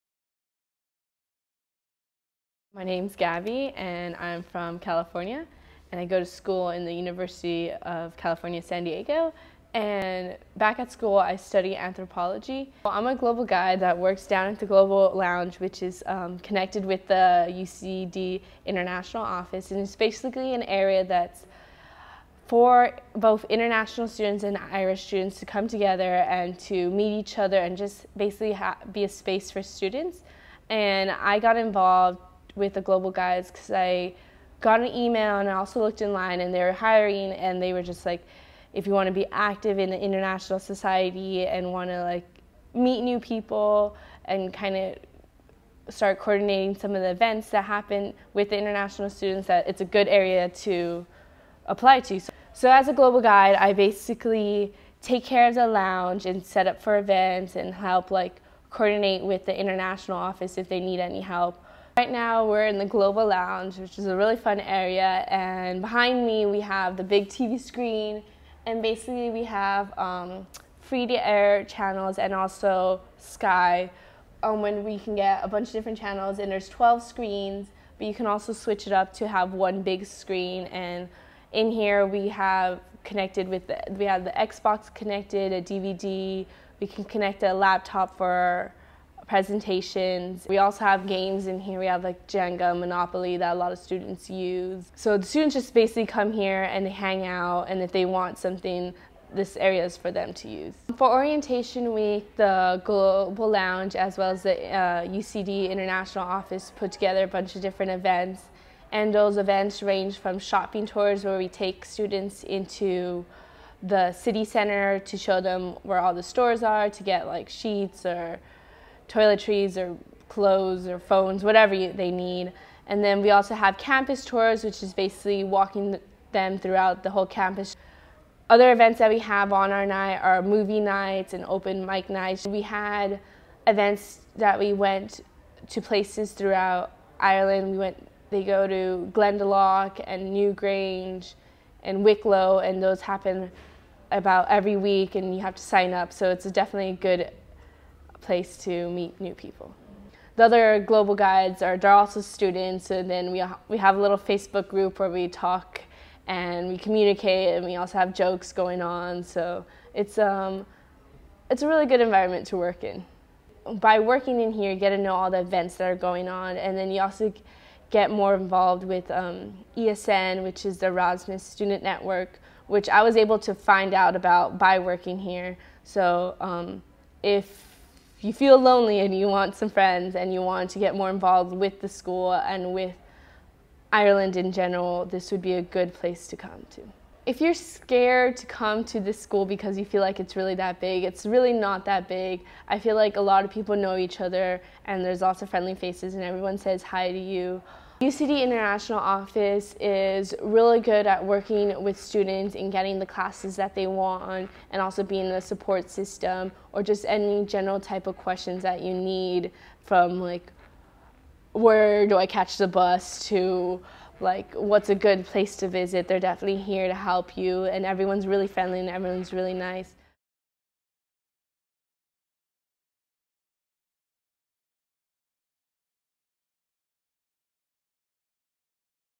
UCD Global Guide
University College Dublin international student Gabby speaks about her role as a Global Guide in the UCD Global Lounge.